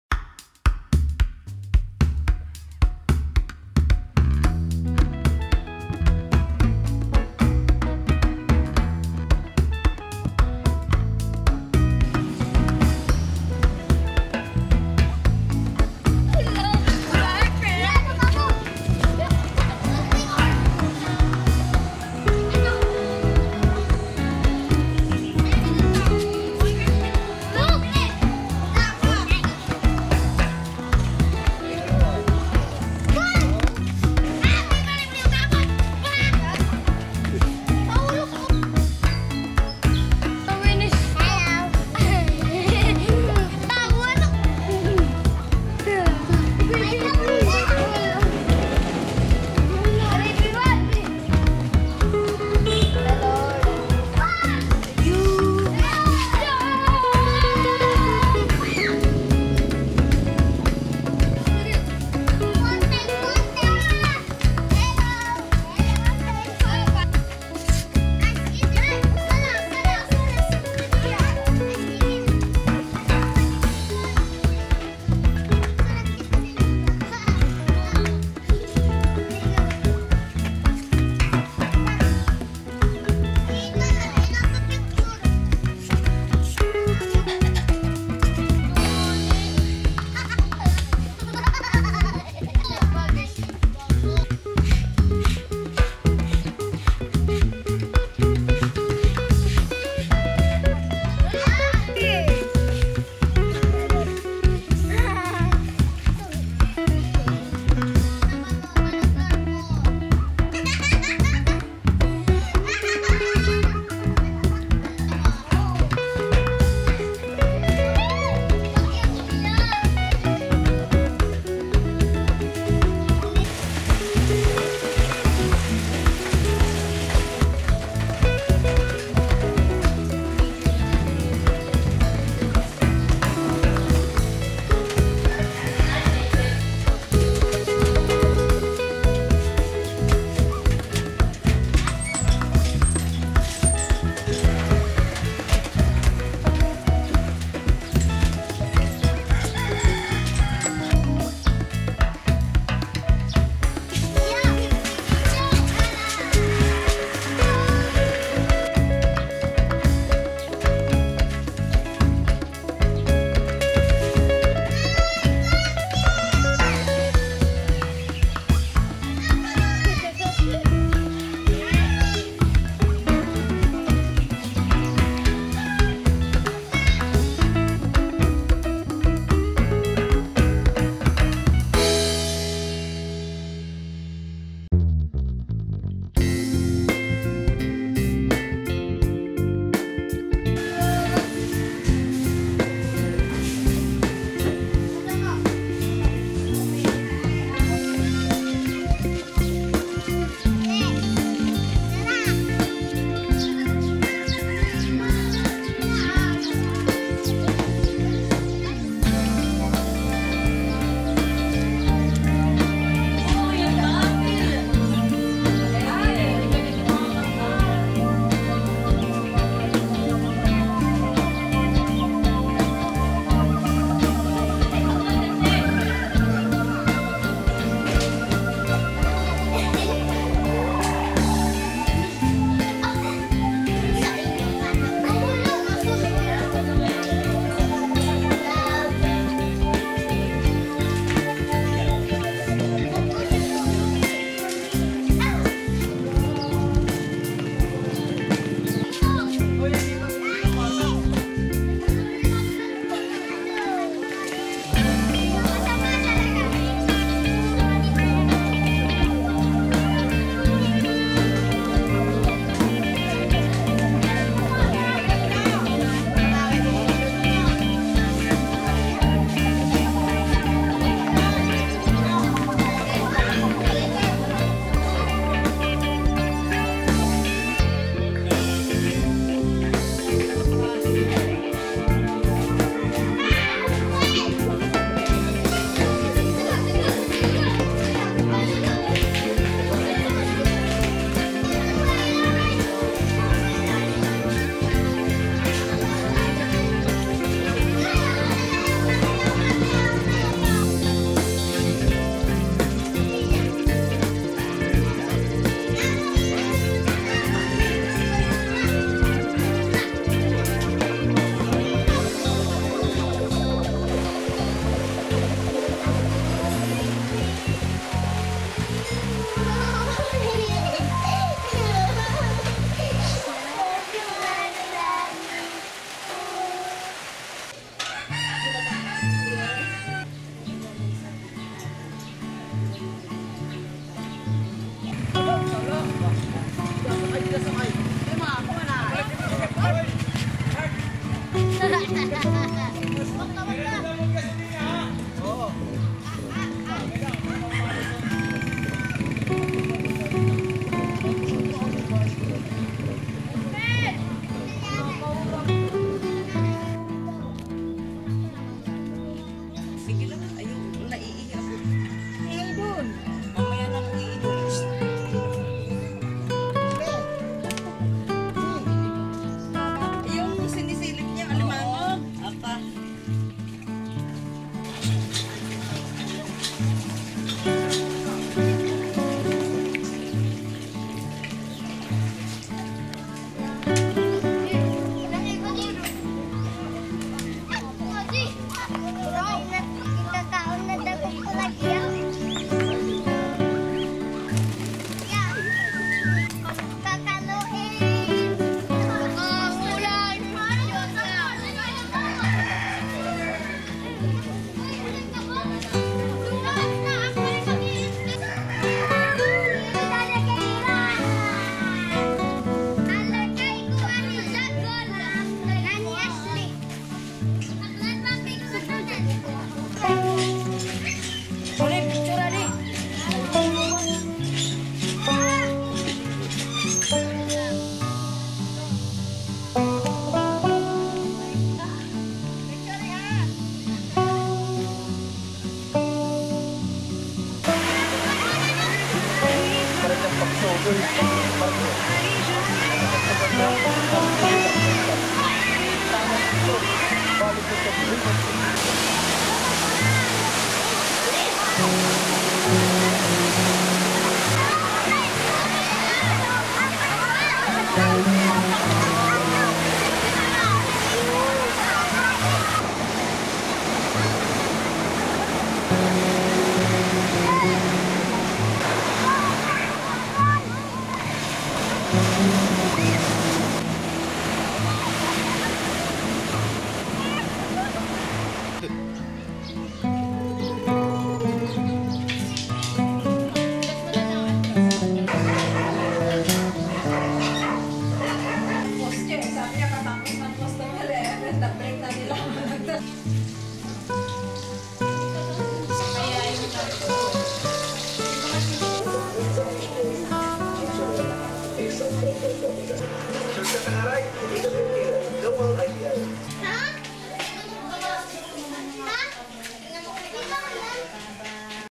self made fun 2018
Philippines kids have fun without expensief toys .